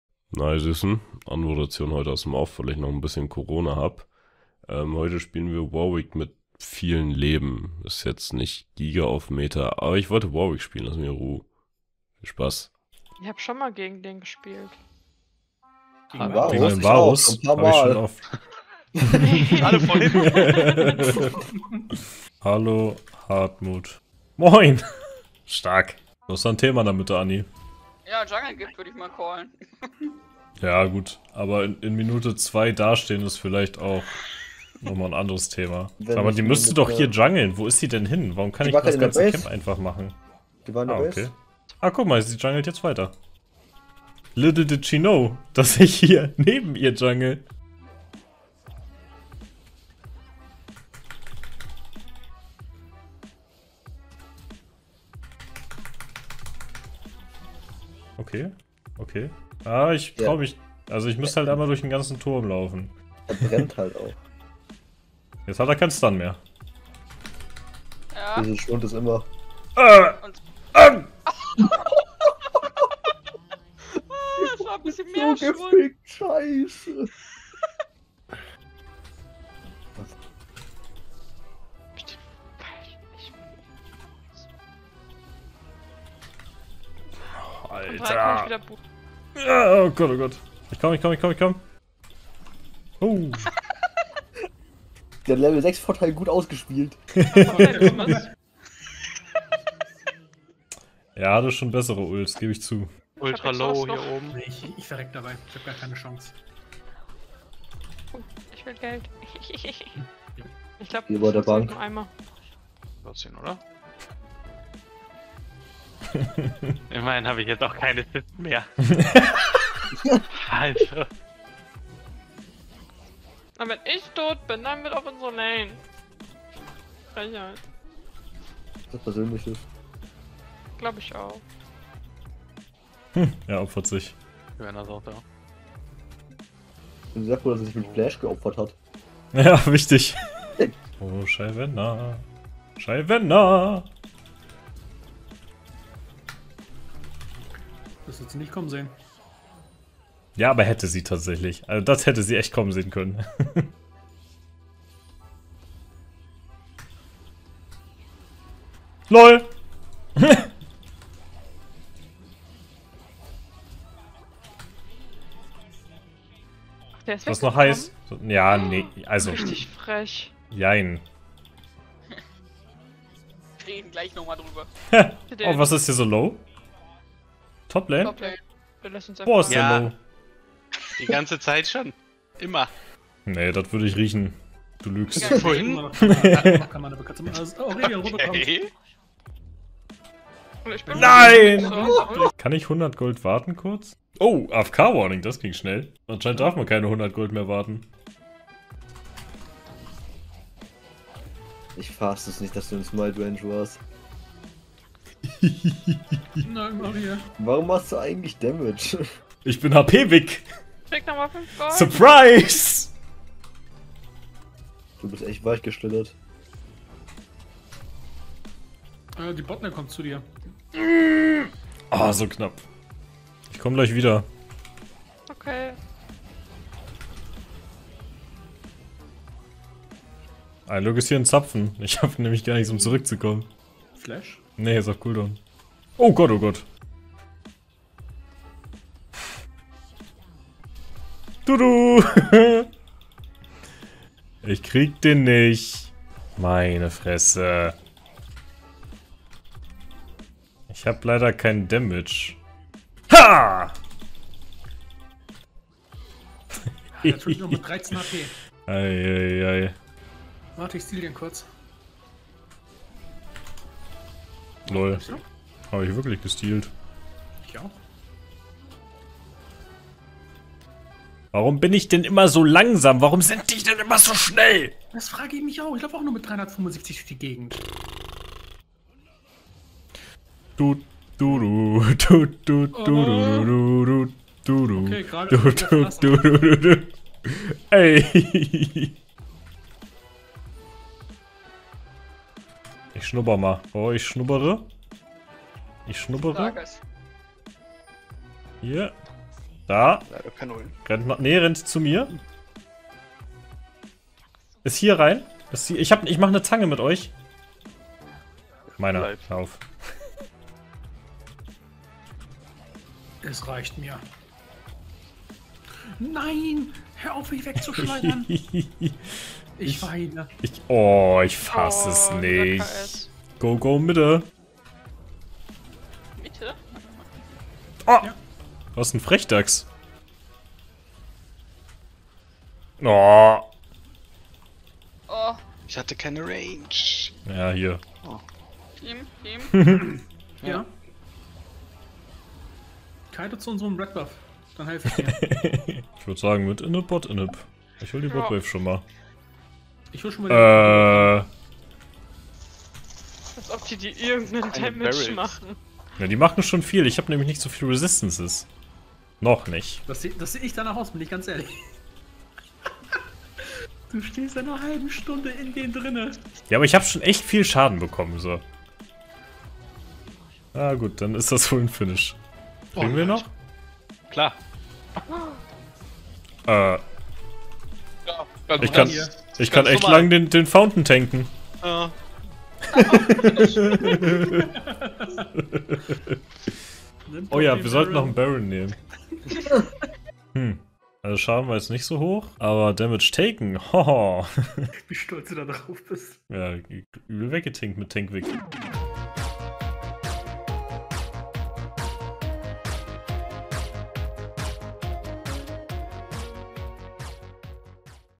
HPWick für mehr Sicherheit
Offmeta Warwick ist schwer, wenn alles auf ihm funktioniert.

┕ Neuster Member: Julian Weghaupt - Gold

🔖KAPITEL
0:00 - Intro
0:13 - Ingame
6:30 - Stats